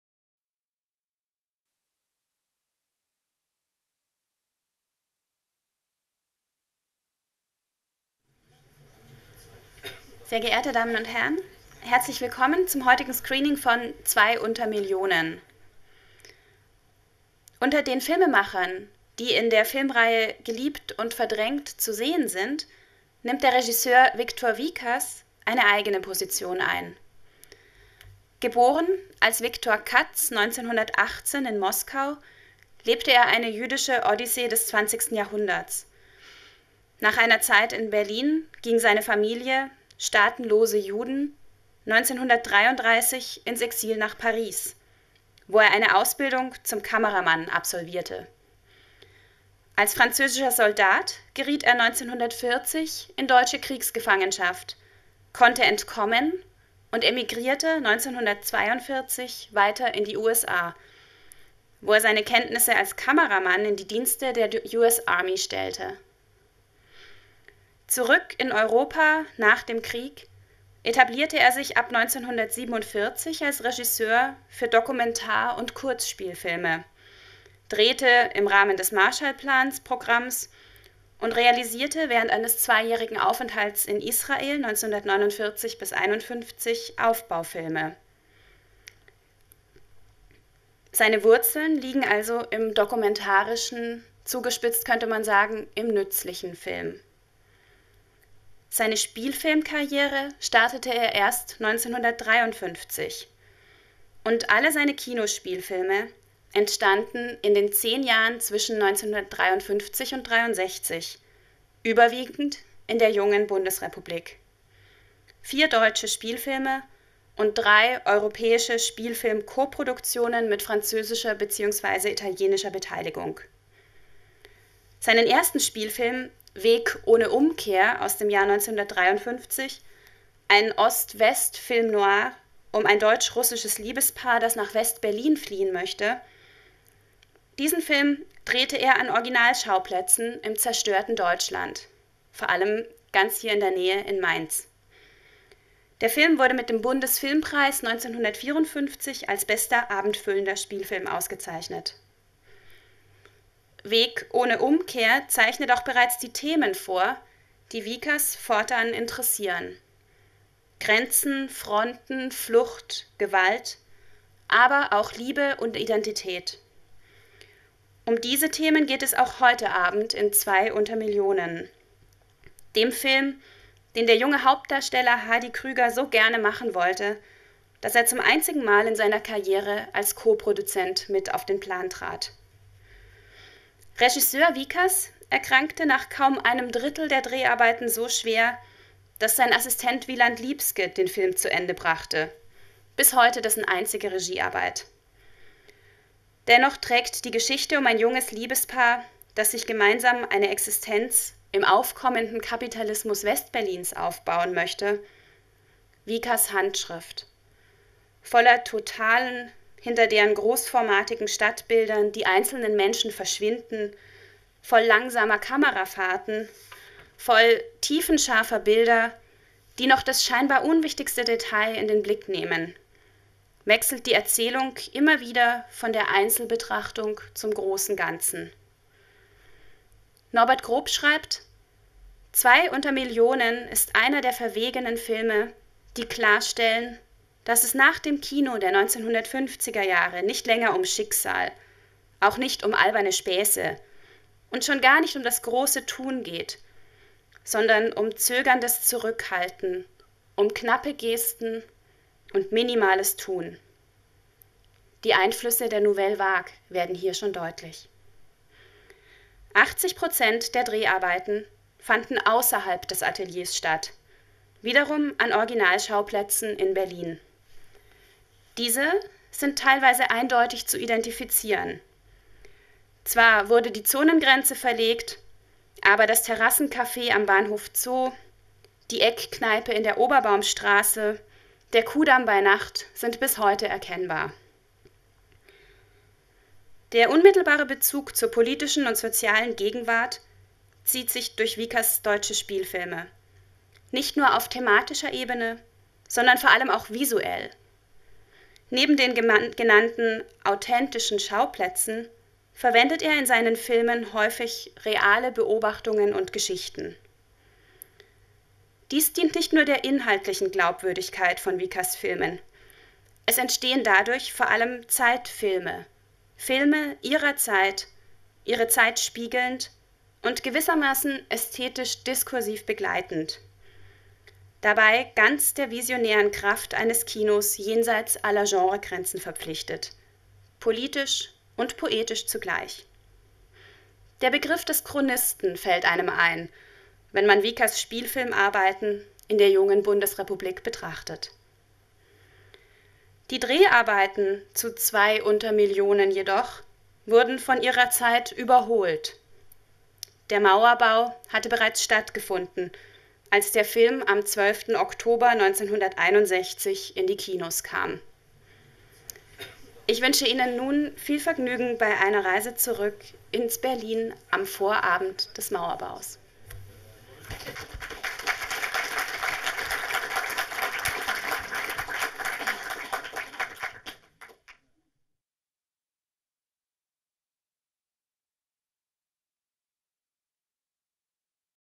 ZWEI UNTER MILLIONEN (BRD 1961) // Vortrag von Stefanie Plappert
GELIEBT UND VERDRÄNGT - Das Kino der jungen Bundesrepublik Deutschland von 1949 bis 1963
Retrospektive zum bundesdeutschen Nachkriegskino

Einführung:Stefanie Plappert (Deutsches Filminstitut)

ZWEI UNTER MILLIONEN
BRD 1961. R: Victor Vicas & Wieland Liebske
D: Hardy Krüger, Loni von Friedl, Walter Giller. 96 Min. 35mm
Noch ist die Grenze zwischen der BRD und der DDR offen, noch gibt es keinen antiimperialistischen Schutzwall, noch ist es nicht ganz so schwer, mit entsprechender Hilfe Rostock illegal in einem Lastwagen zu verlassen, Richtung Westberlin. Und nun sitzt Christine da, umgeben von Gütern, die sie sich nicht leisten kann, und versucht sich mit ihrem Fluchthelfer-Liebsten Karl über Wasser zu halten … Eine neorealistisch infizierte Straßenfilm-Preziose, in der man die wirtschaftswunderreich-korrupte BRD und Westberlin ziemlich exakt so zu sehen bekommt, wie es die DDR immer wieder gesagt hat – nur poetischer und zugleich rauer.

Veranstaltung im Kino des Deutschen Filmmuseums, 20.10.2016